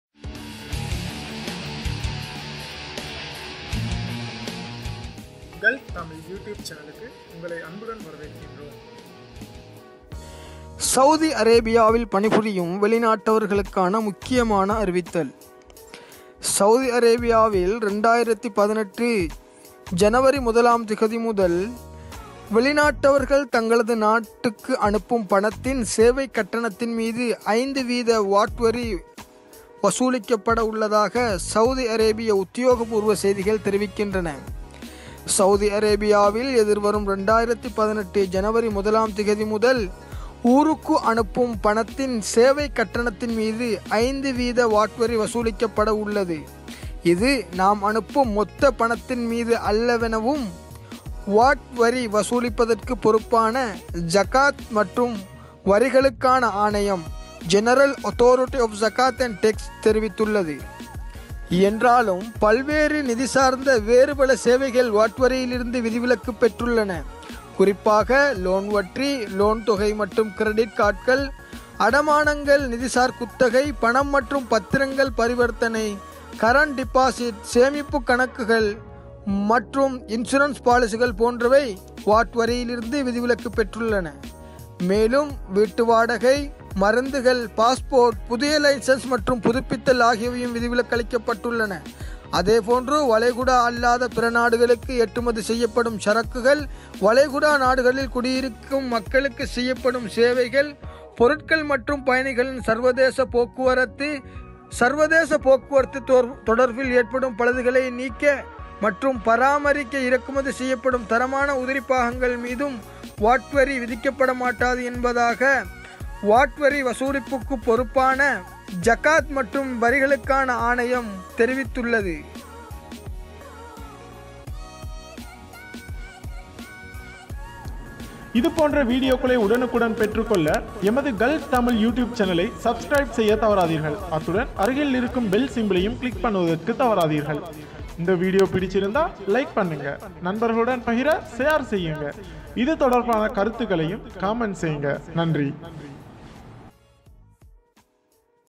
Saudi Arabia will implement 5% VAT on money transfer fee from January 2018
சவுதியில் எதிர்வரும் 2018 ஜனவரி.1 முதல், அனுப்பும் பணத்திற்கு 5 % VAT கட்டணம் அமூல்!